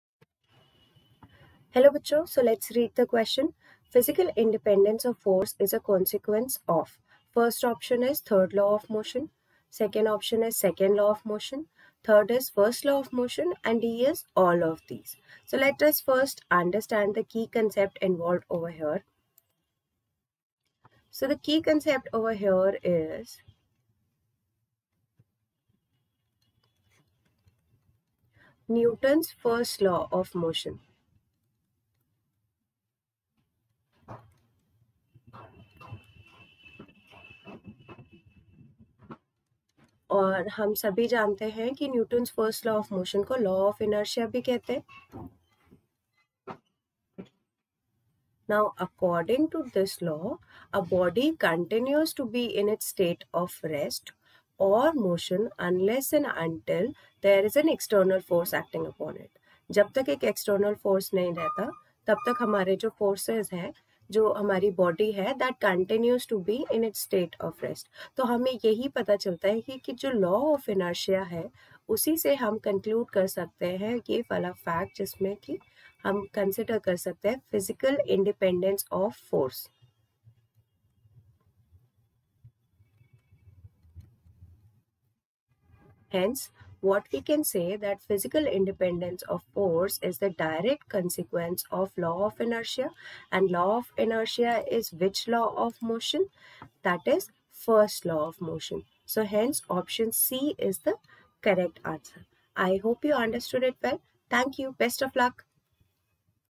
Physical independence of force is a consequence of (a) third law of...
Physical independence of force is a consequence of
(a) third law of motion
(b) second law of motion
\( \mathrm{P} \)
W
(d) all of these laws
(1991)